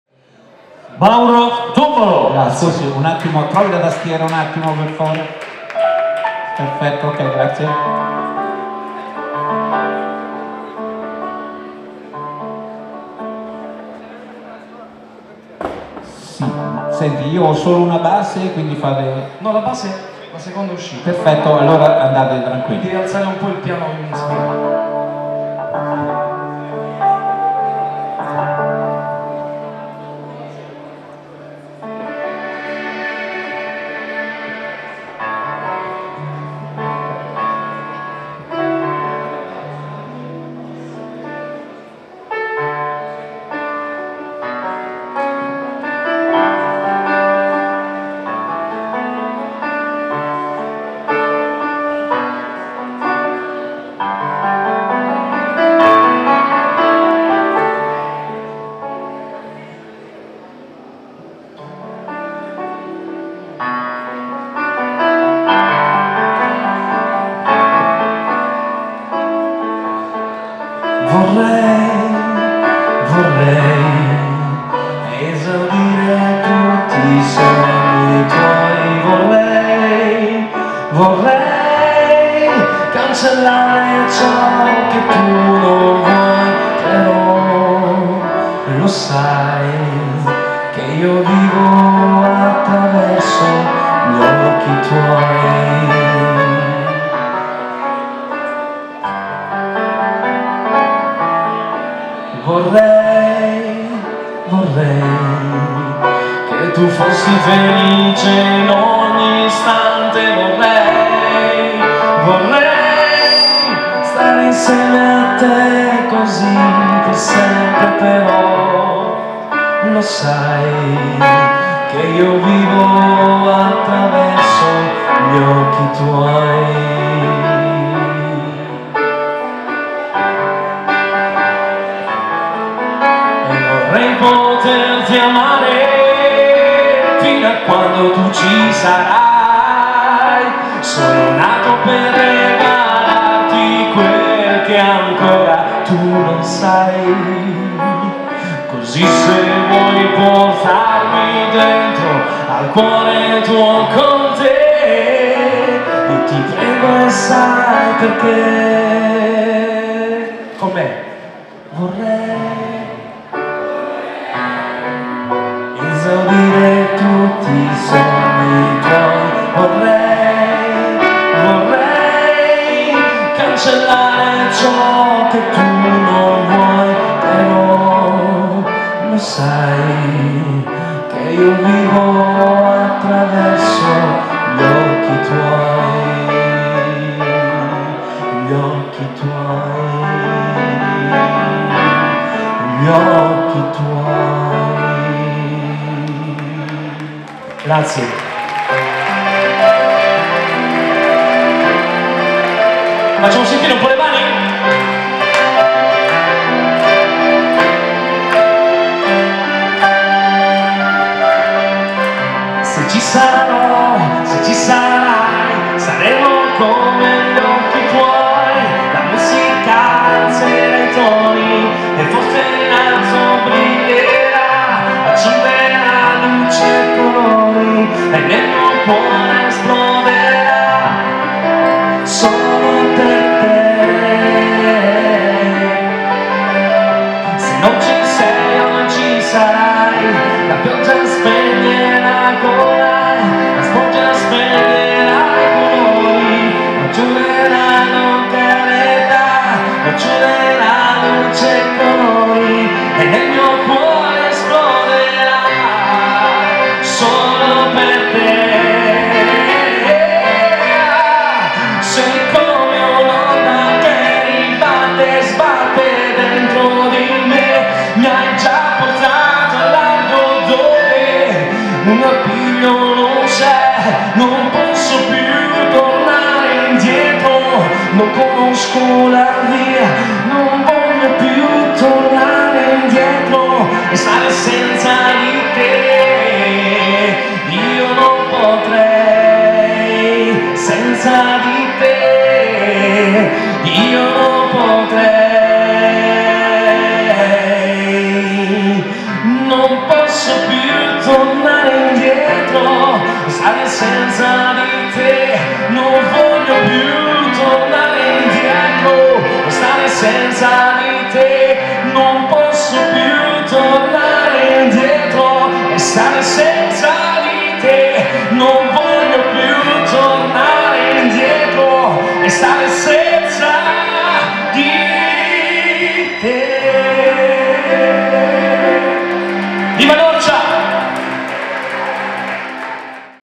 Nero Norcia 2017 - Mauro Tummolo
Il Tuo Canto Libero in Tour 2017 con Green Stage Radio - Talenti in Tour collegato al Bussola Casting c/o Cinecittà Studios.
Umbria nel Cuore a Norcia. Condotto da Giorgio Contilli.
Video Pietro Zambotti